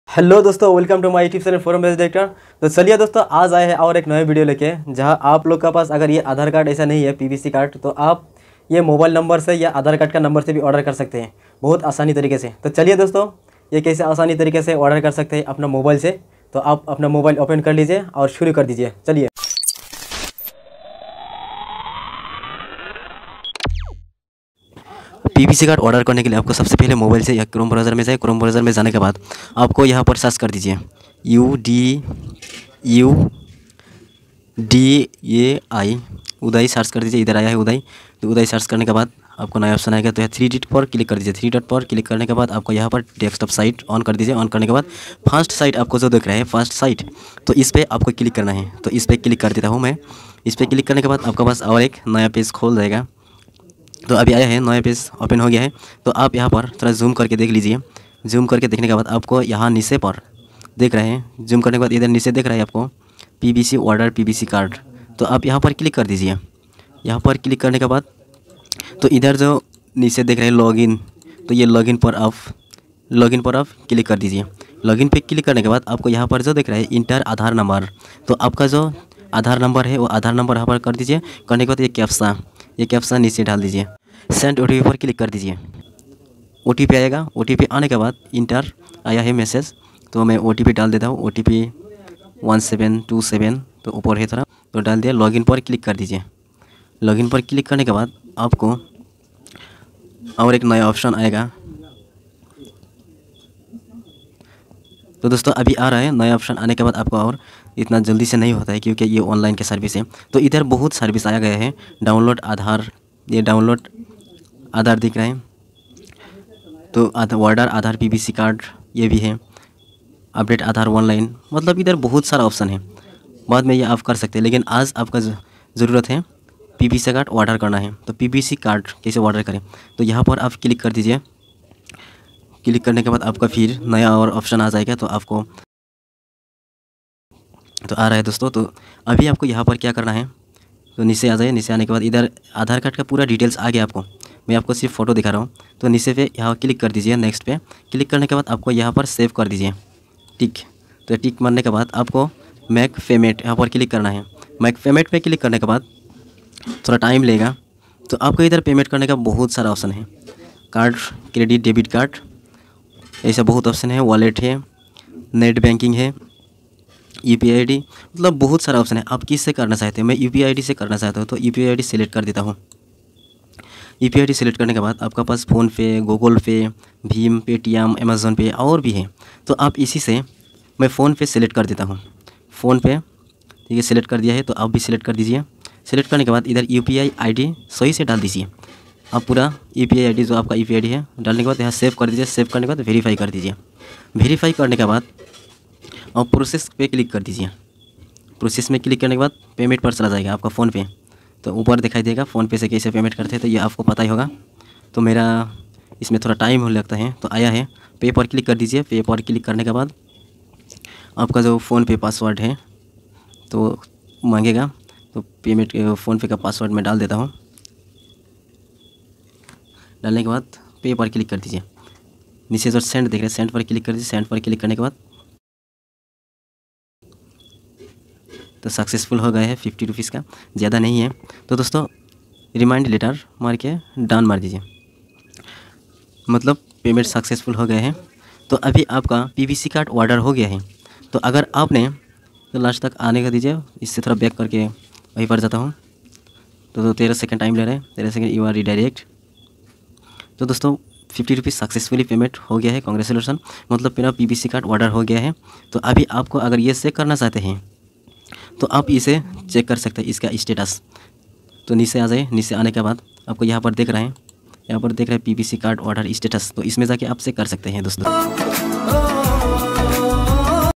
Aadhaar PVC Card Order Online : मोबाइल नंबर से ऑर्डर करें पीवीसी कार्ड,ऐसे करें अप्लाई
Hey dost ::👌

Supoort me